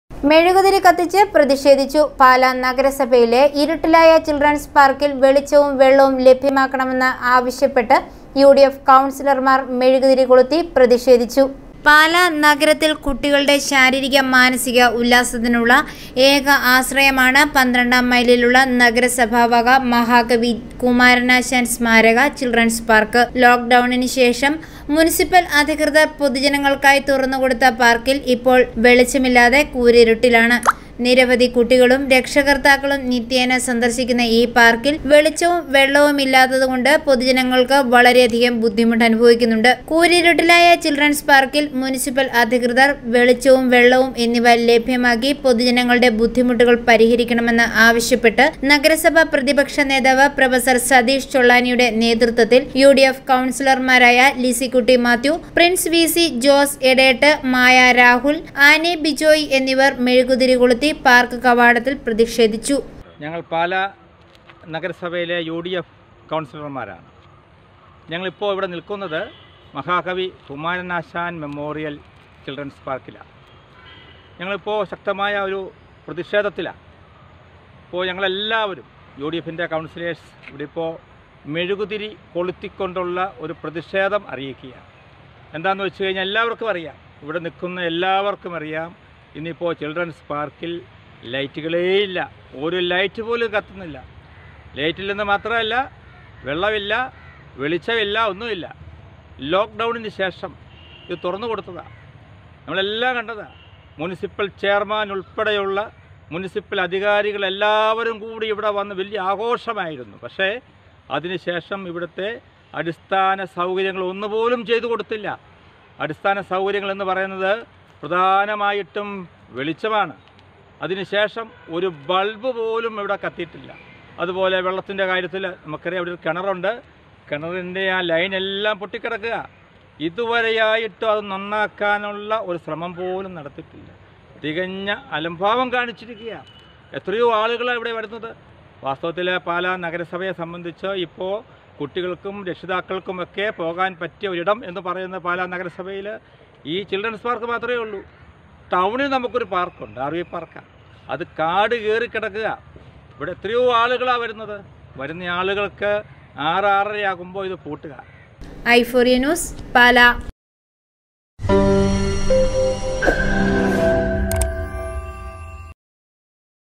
ചില്‍ഡ്രന്‍സ് പാര്‍ക്കില്‍ വെളിച്ചവും വെള്ളവും ലഭ്യമാക്കണമെന്ന് ആവശ്യപ്പെട്ട് യുഡിഎഫ്
മെഴുകുതിരി കത്തിച്ച് പ്രതിഷേധിച്ചു. പാല നഗരസഭയിലെ ഇരുട്ടില്‍ ആയ ചില്‍ഡ്രന്‍സ് പാര്‍ക്കില്‍ വെളിച്ചവും വെള്ളവും ലഭ്യമാക്കണമെന്ന് ആവശ്യപ്പെട്ട് യുഡിഎഫ് കൗണ്‍സിലര്‍മാര്‍ മെഴുകുതിരി കൊളുത്തി പ്രതിഷേധിച്ചു.